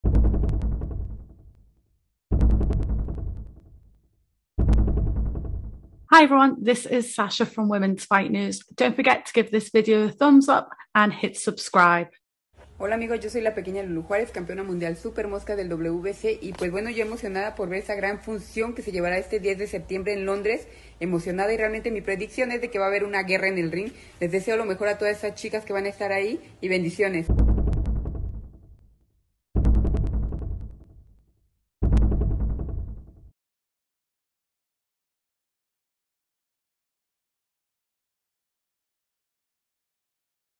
Lourdes Juarez - Sky Sports Boxing predictions - Shields-Marshall + Mayer-Baumgardner.
Sky Sports Boxing alongside BOXXER, Top Rank and ESPN will broadcast the Claressa Shields - Savannah Marshall Undisputed Super Middleweight   fight. Co-main will see Mikaela Mayer - Alycia Baumgardner fight to be crowned unified super-featherweight world champion. The historic night of boxing will see 11 female bouts take place at the O2 Arena, London on September 10th 2022. Lourdes Juarez gives her predictions!

🥊 Claressa Shields - Savannah Marshall fighting on Sky Sports to become undisputed!

🥊 BOXXER will host the super-featherweight unification fight of Mikaela Mayer - Alycia Baumgardner